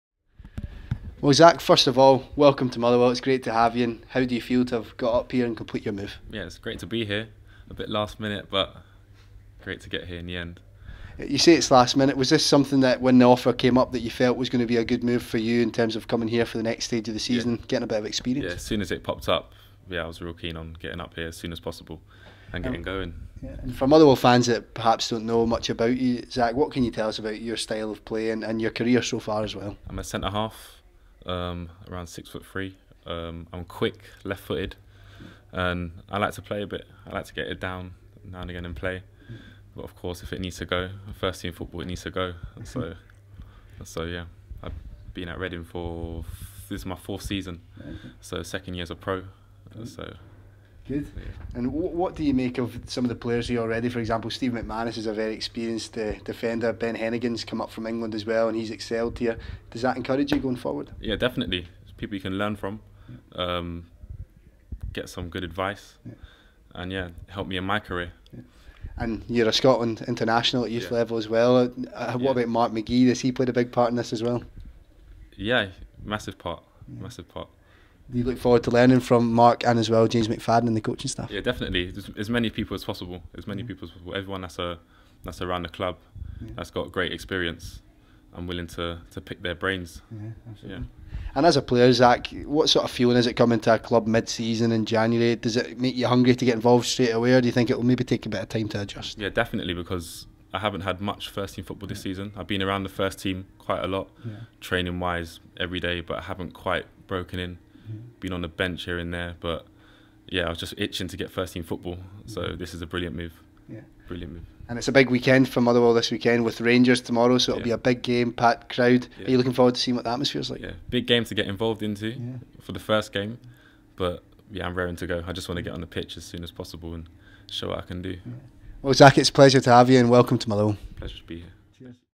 Zak Jules joins on loan from Reading
Scotland U21 defender Zak Jules has put pen to paper on a loan deal until the end of the current 2016/17 season.